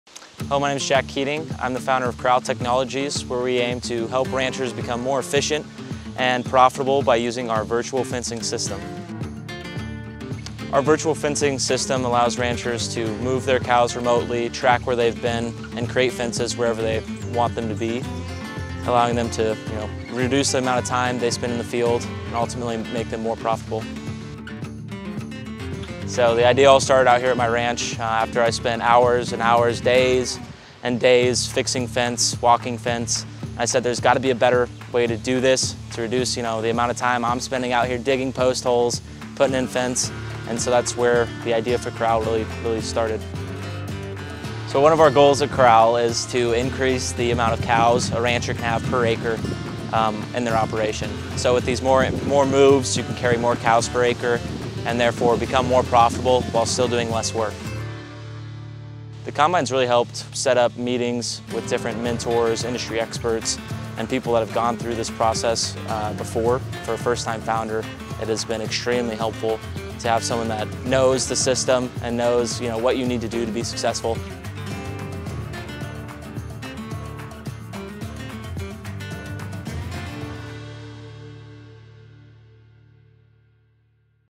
Corral Technologies | Virtual Fencing for Livestock Producers
Saving You Time, Money, and Headaches with Corral Technology -

Corral has advanced technology that will improve pasture utilization. This technology will virtually cross fence any pasture decreasing the number of rented acres and reduce the amount of fencing needed every year. With the option of using a cell phone, computer, or tablet, anyone can easily manage their ranch from almost anywhere. Ranchers will have the ability to move, track, and view data on their cows through the Corral software. This allows for the efficient management of any ranch and guides ranchers to reach their goals.

Overall, Corral gives ranchers the tools to create the best possible solution for their ranch.

Learn More About The Combine at nebraskacombine.com